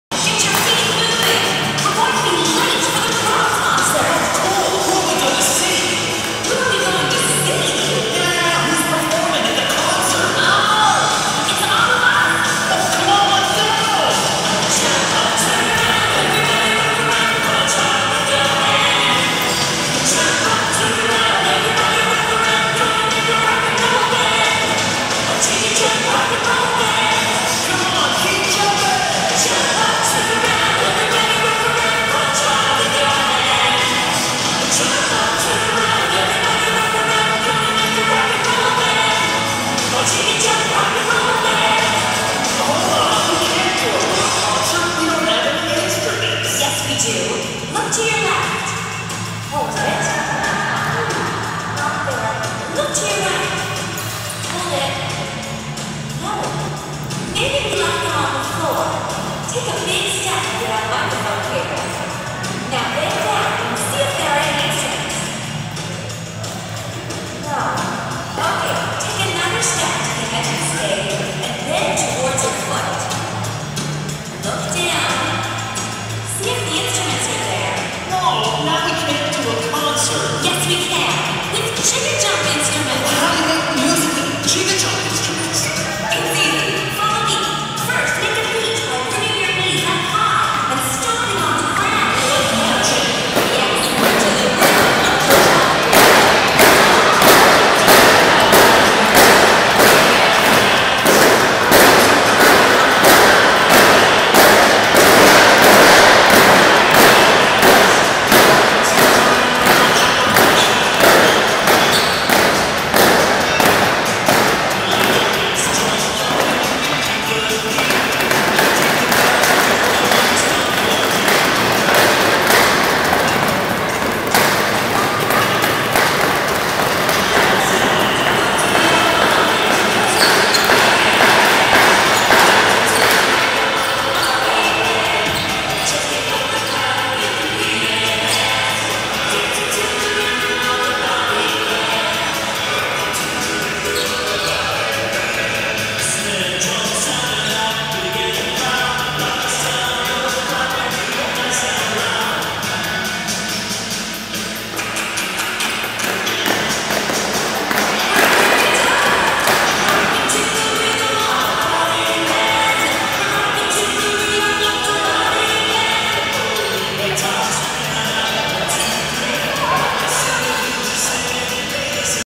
JiggiJump Rock With Props
JiggiJump Rocks using pool noodles, balls, discs, and hoops (as telescopes, magnifying glasses, glasses, drums, drumsticks, and guitars.